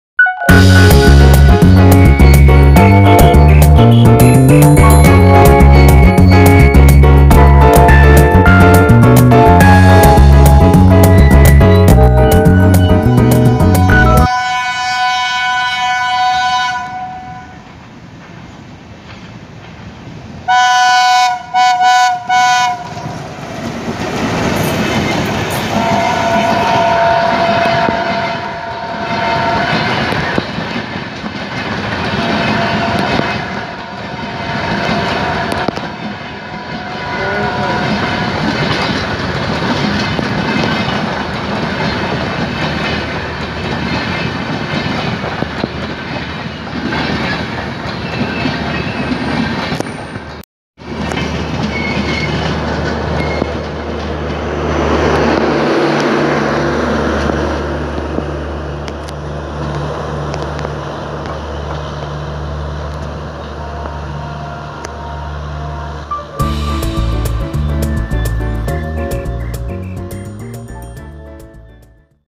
MGS WDM3A 12319 KOLKATA AGRA CANT SF EXPRESS arrived FATEHGARH speed 30-40 kmph 3 hour 30 min late
Wdm3a 12319 slowly arrived at fatehgarh  Because the work was running on 1 number platforms, so the train went slow